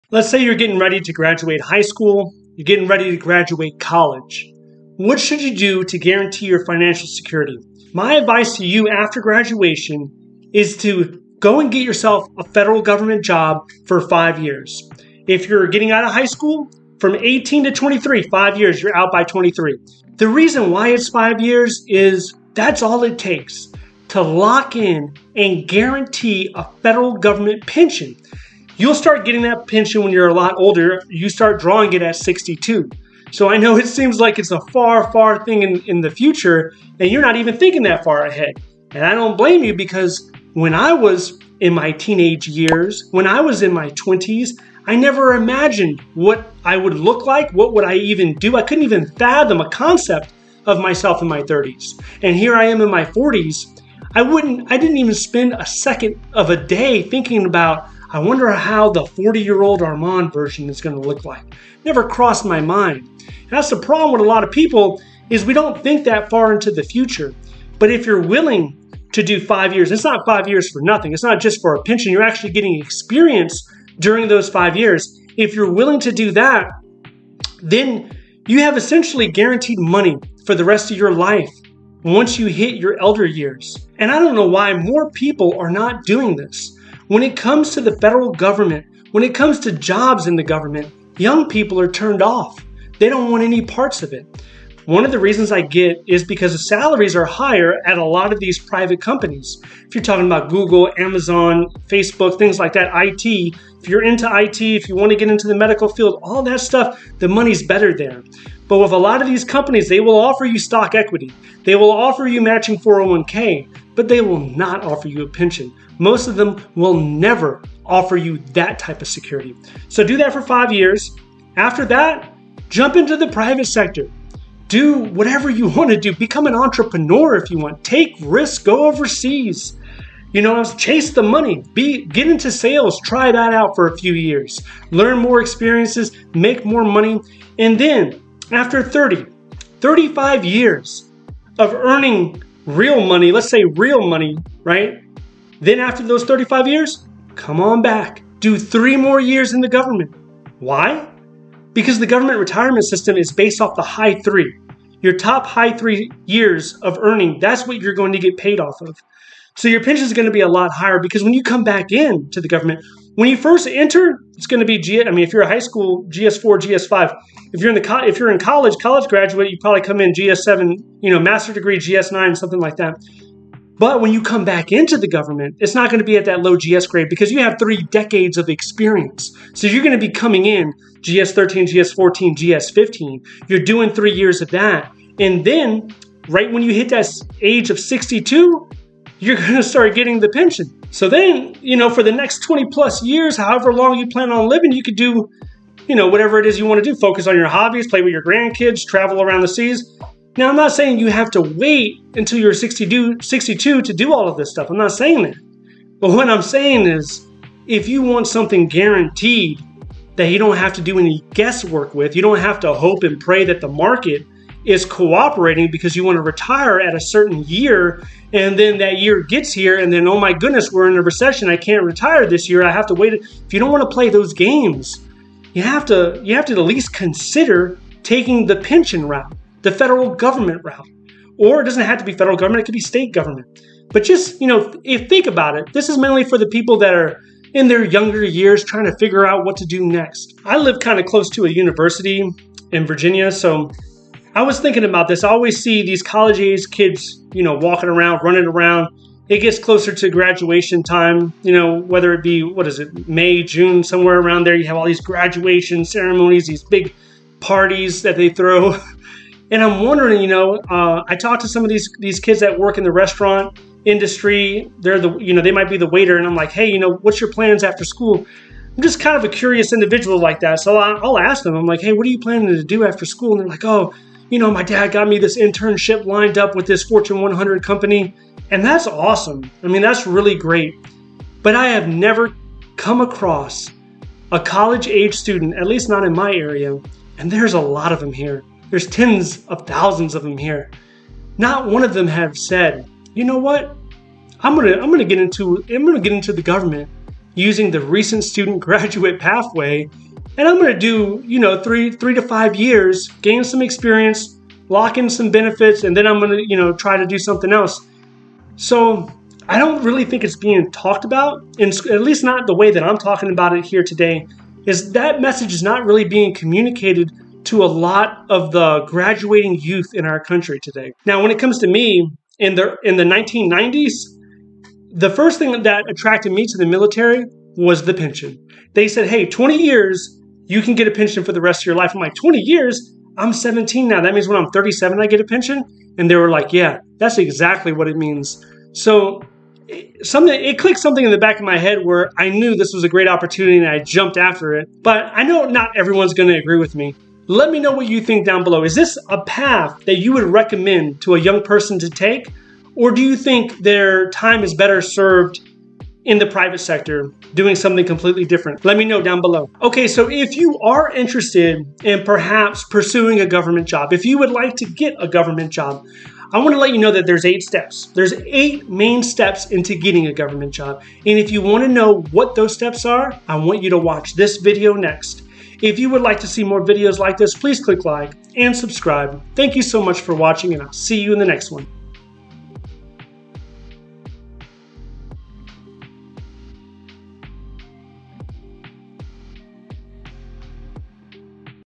Why THIS Should be Your 1st Job After Graduation
You can make decisions after graduating from high school or college that could secure your financial future. This can result in money coming in for the rest of your life. The decision you need to consider is getting a federal government job. If you do that for five years,  you will lock in a federal pension that you could start drawing at 62 years old or earlier. In this video, I discuss the concept behind securing a government job after graduation. Have you started to apply to a government job? Let me know down below.